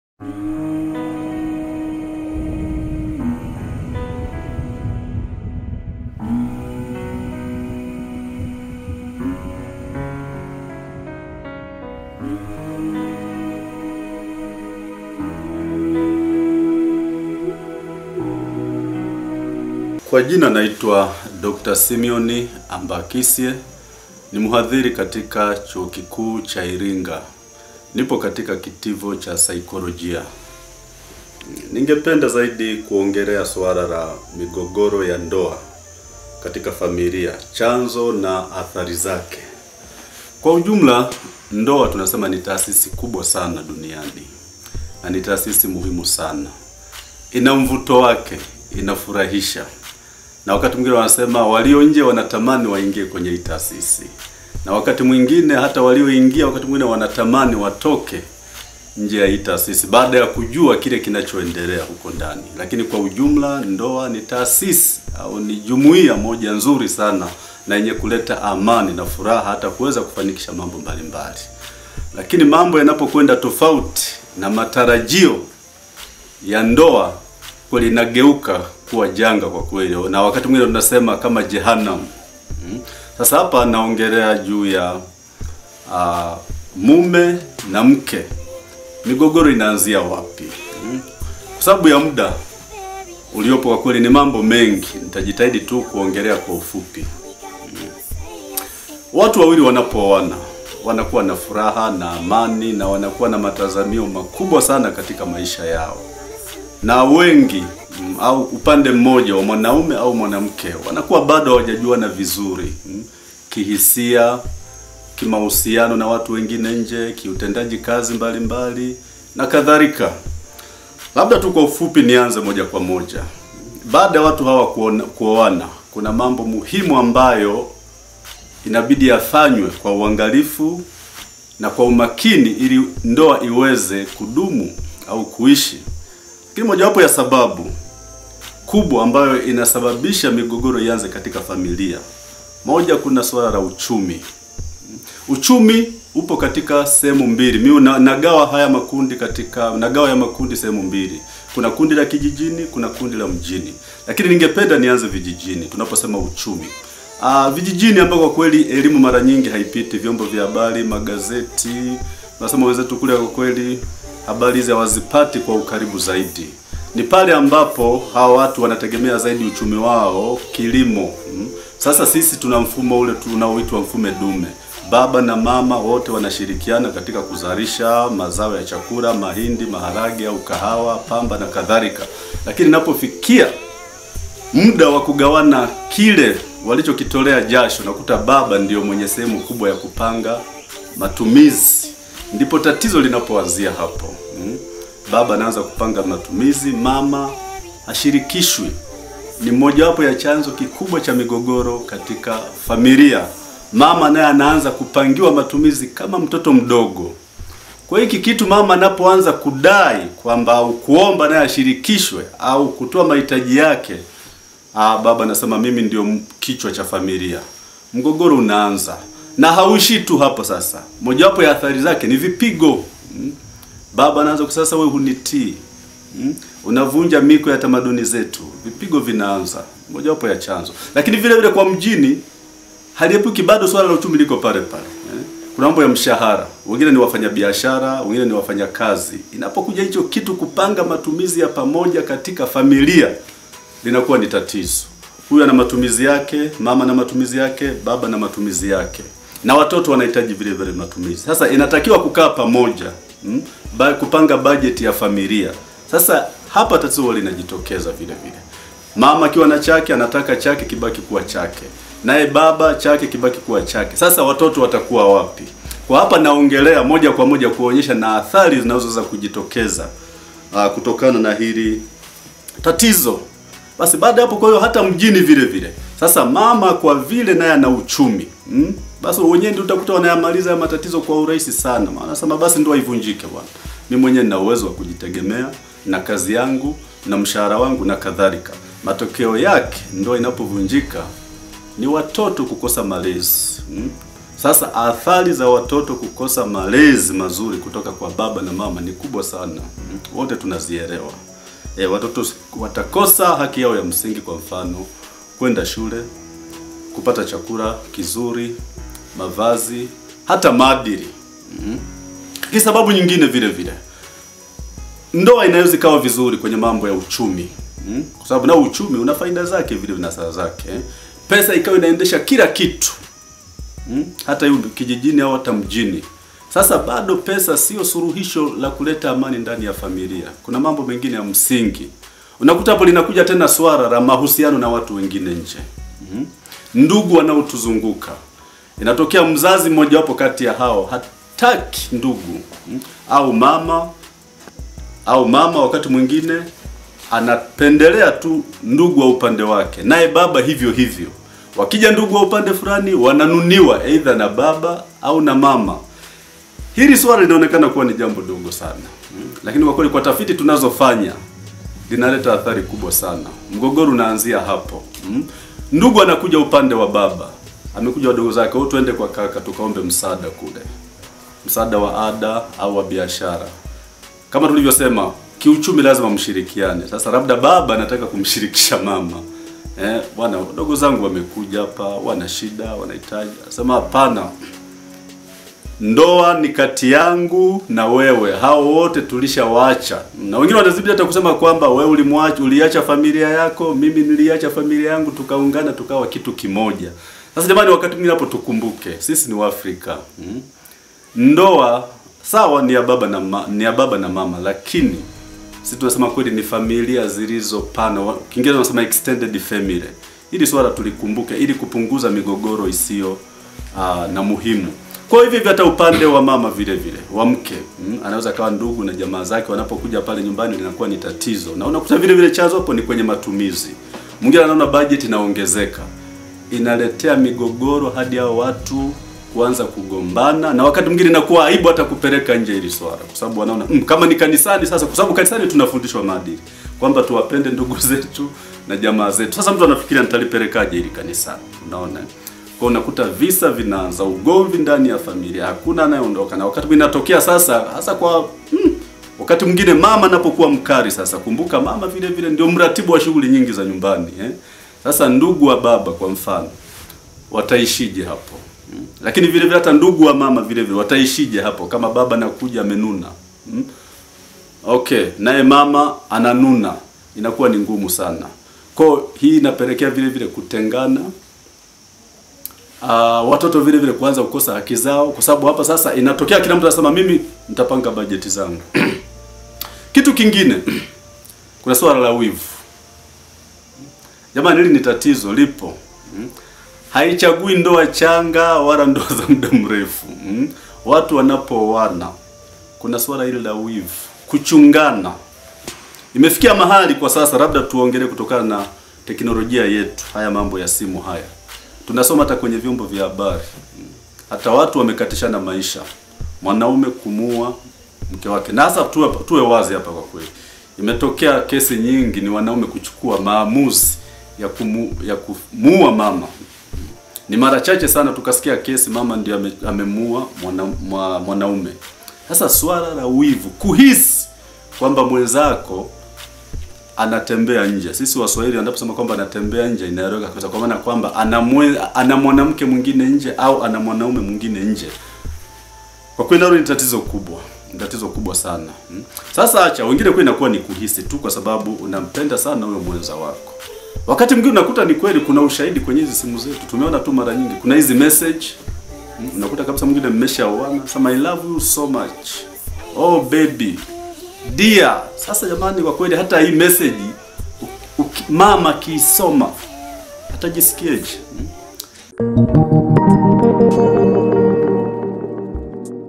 AFYA YA AKILI
CHANZO CHA KUVUNJIKA KWA NDOA NA MADHARA YAKE